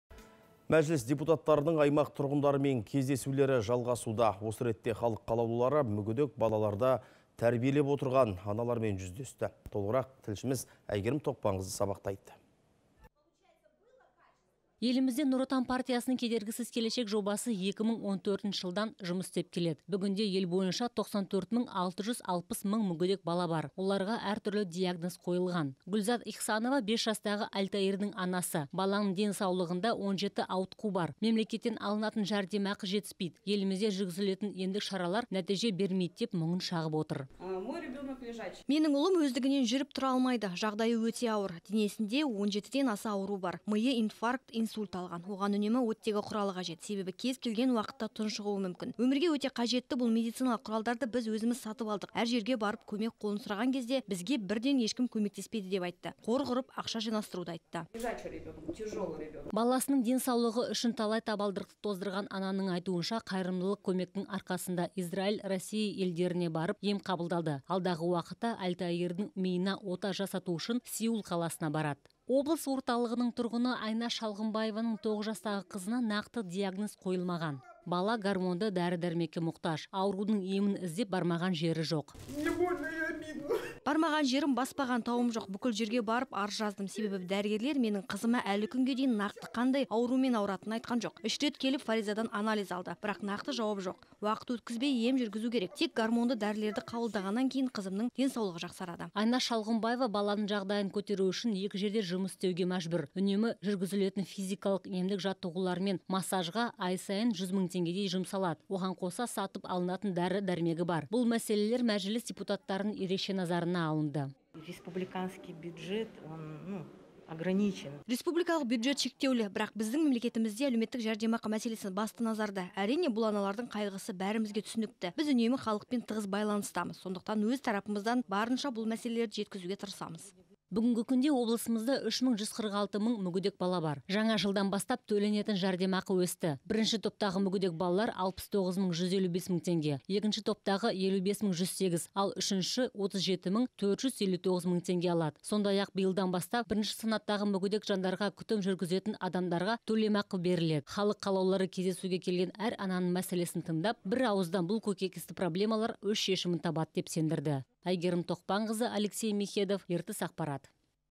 Мүмкіндігі шектеулі балаларға көмек көрсетілуі тиіс
Мәжіліс депутаттарының  аймақ тұрғындарымен кездесулері жалғасуда. Осы ретте халық қалаулылары мүгедек балаларды тәрбиелеп отырған аналармен жүздесті.